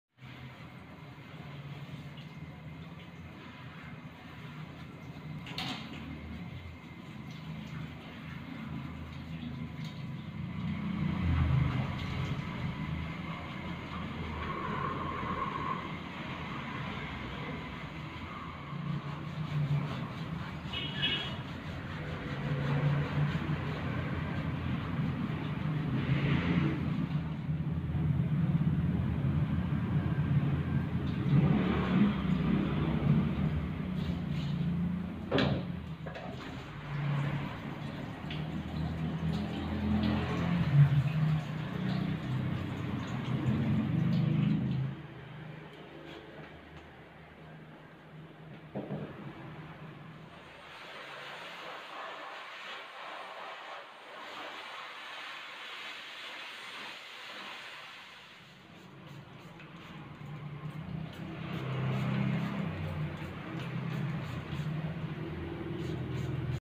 fullbath Chow Chow Dog Grooming
Non-Sporting many pet dogs are selected from this category.
While this group may well have performed some specific task in the past, today they are more commonly used as companions for their owners.

Chow Chow - A very ancient breed, the chow chow is powerful, imposing animal with a soft, dense coat, An excellent guard dog, it also responds well to training.

Height: 46-56cm
Weight: 25-40kg